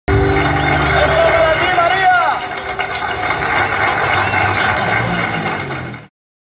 Cadillac Shadow ESTO ES PARA TÍ MARÍA💃 Dulce Harleey 🎵Mancha Cutom Club 24 concentración Socuéllamos
Dedicatoria a MARÍA DULCE HARLEEY que estaba allí presente :)
Concentración de moteros/as, en Plaza de Toros de Socuéllamos, organizado por Mancha Custom Club!
Actuación de Cadillac Shadow junto con nuestras amigas de La Dulce Harleey.
Gran noche inolvidable de Rock And Roll en la fiesta 24 concentración custom.

Os mando un saludo de parte de Cadillac Shadow, Myky Roldán y Miguel Rockrocker